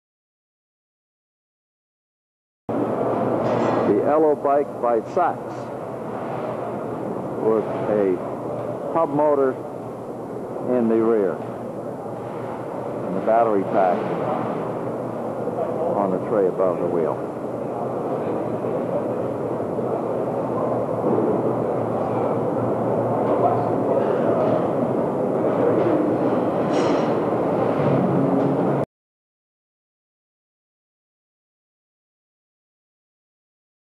Intercycle Electric Bicycle exhibits - episode 1
This is one of a series of videos documenting electric bicycles on exhibit at Intercycle in Cologne Germany 1995

"Ello"(spelling?) by "Sachs"(spelling?) with an electric hub motor in the rear wheel.